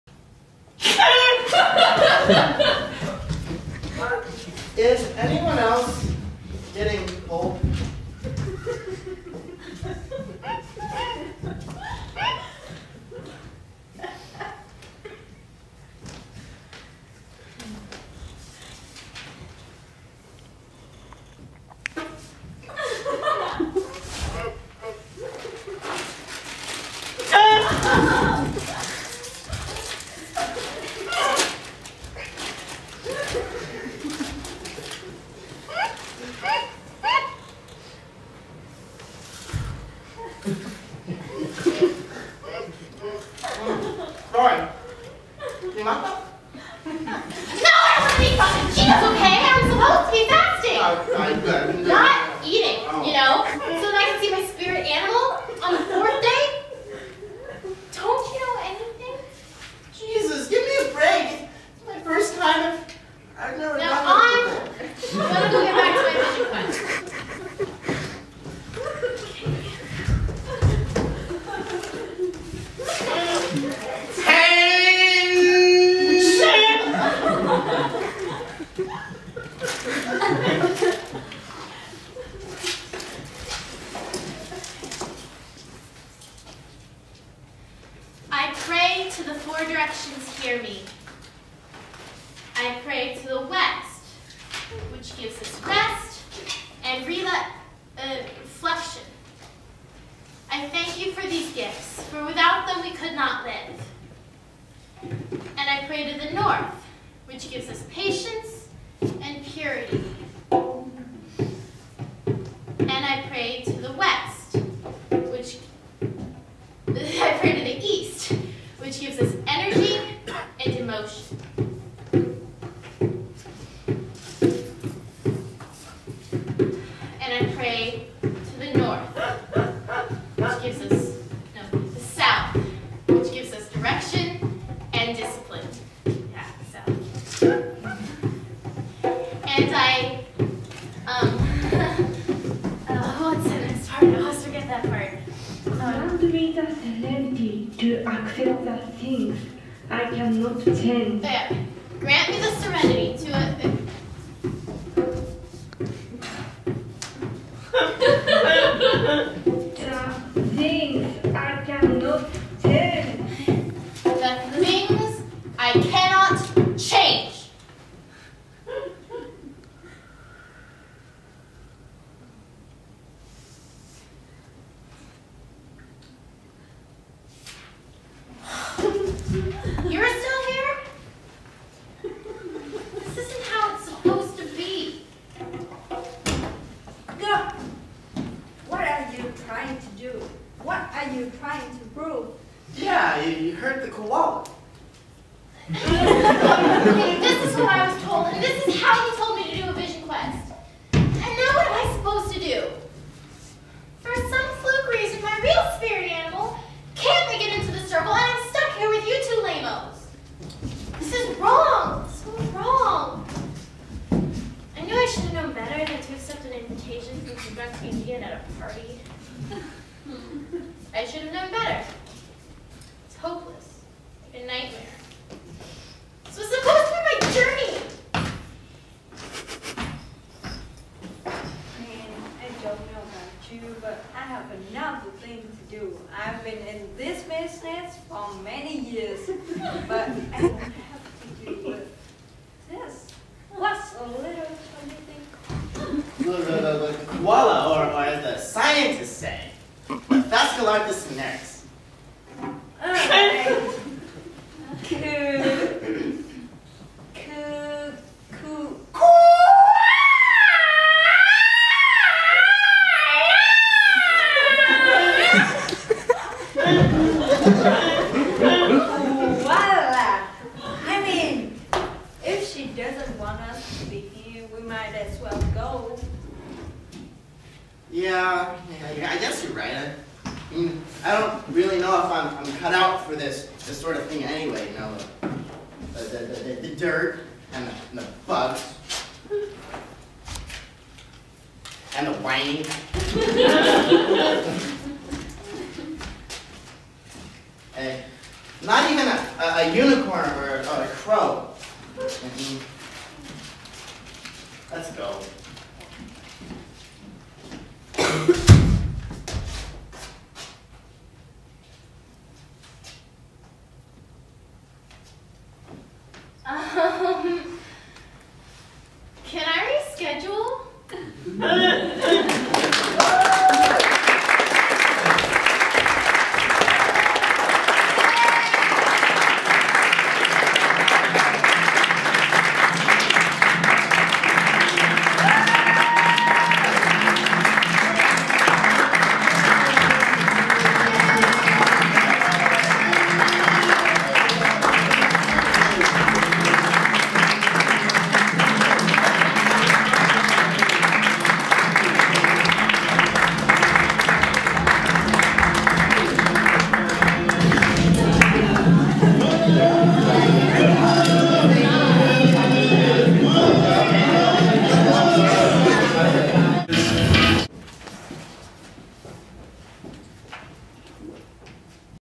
Teepee Girl part 2
Teepee Girl embarks on a spiritual journey to find herself, but instead finds something quite different then she'd  ever  imagined.

A 10min Play Written and Directed by Jennifer Mercede and Heather Hawkins

Teepee Girl- Amelia Bear
Squirrel- Nathan Smith
Owl- Ha Sai
Koala- Sean Dugan